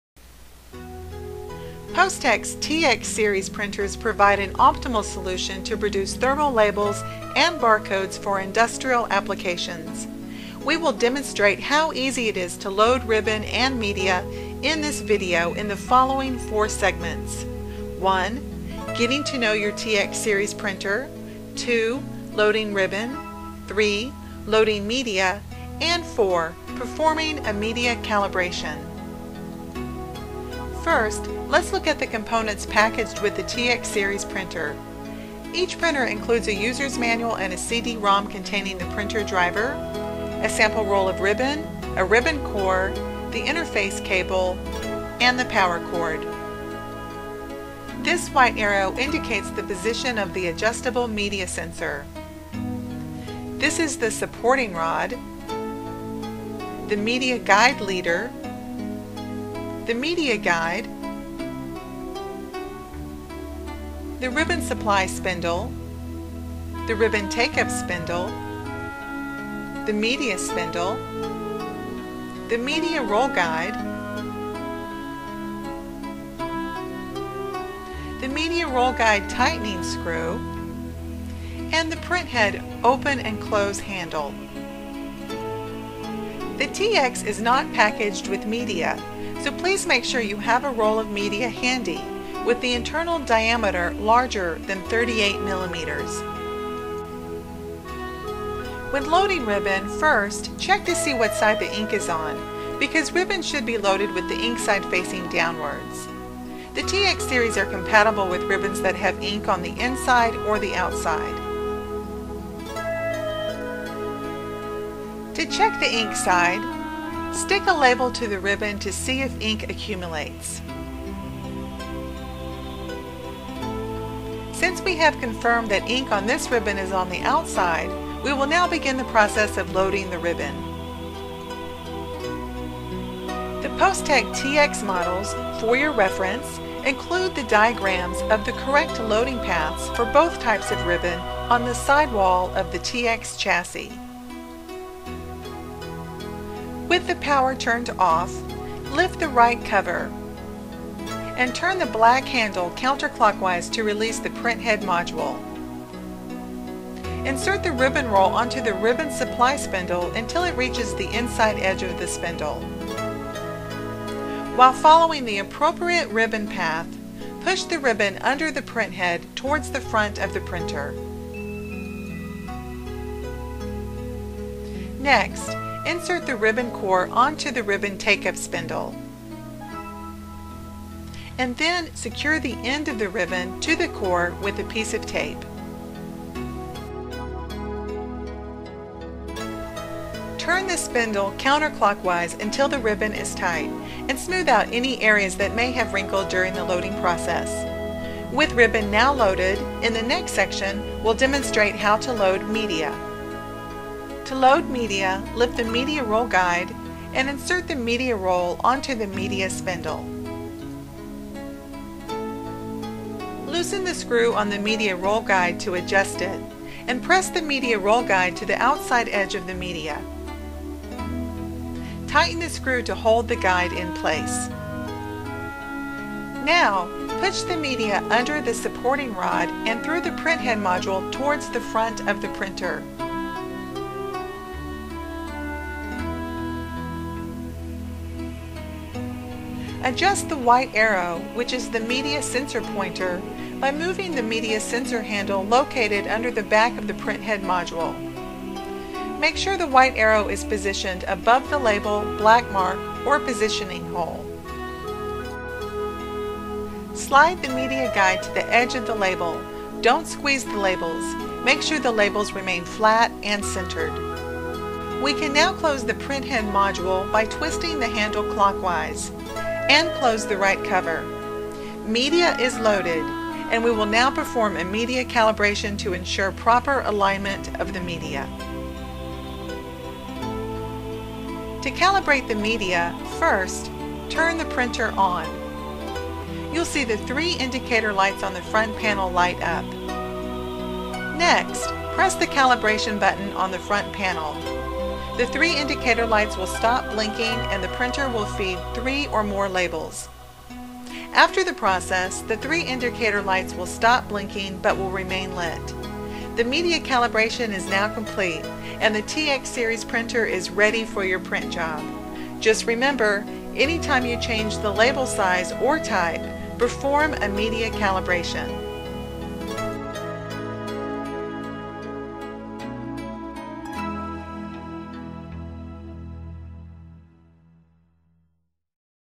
Postek TX Series - Loading Media and Ribbon.wmv
Postek's TX-Series high performance, heavy duty industrial barcode printers surpass performance expectations with remarkable reliability.  This video demonstrates how easy it is to load media and ribbon, and perform a media calibration. Viewers can see the printers metal construction and unique high strength aluminum core, components that give the TX a sturdy and durable stationary platform. For more information on Postek's TX-Series industrial thermal label printers, please visit the Postek